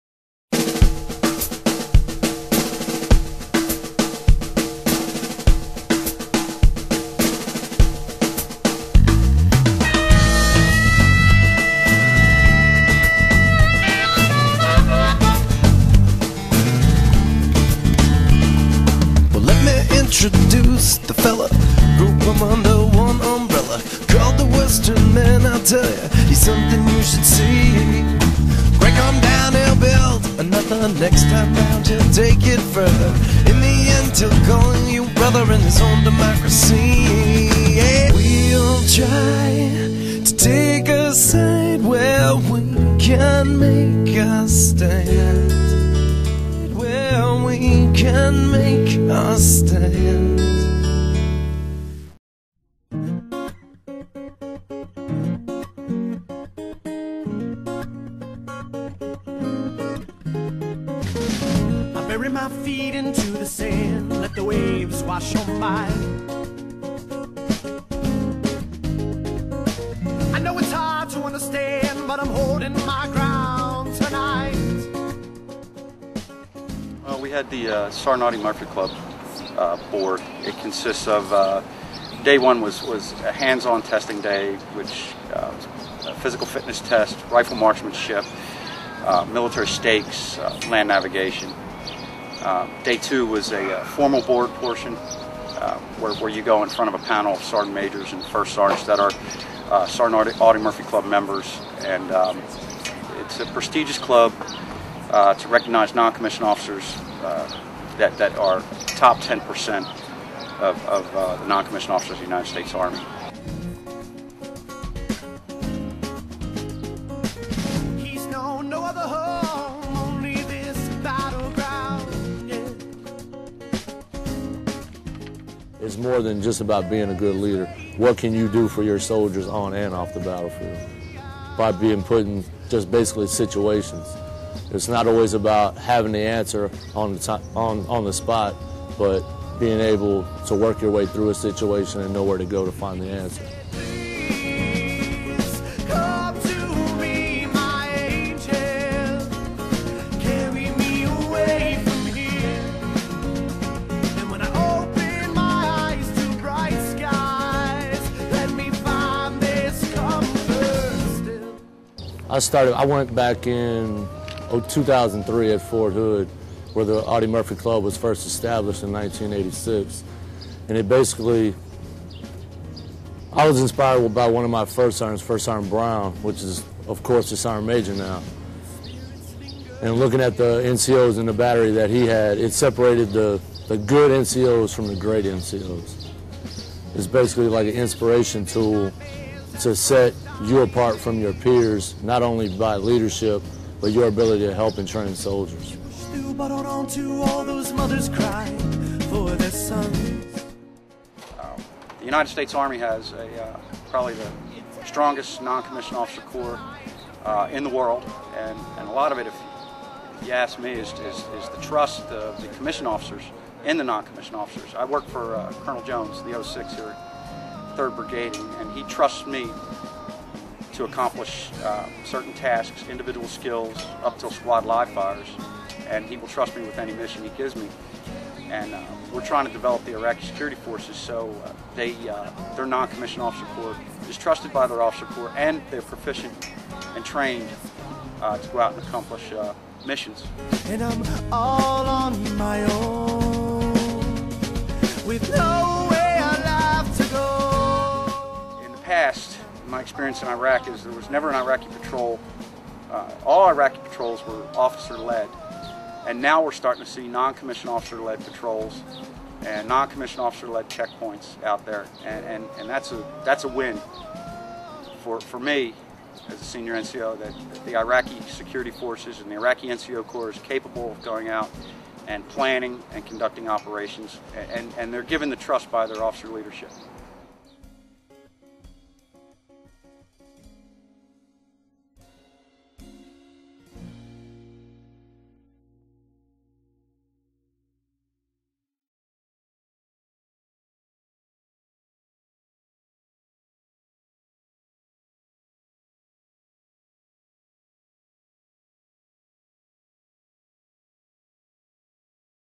HAMMER - Episode 15
Episode 15 of HAMMER follows along as NCO's of the Sledgehammer Brigade vie for membership in the exclusive Sergeant Audie Murphy Club.  Episode music courtesy Josh Albright.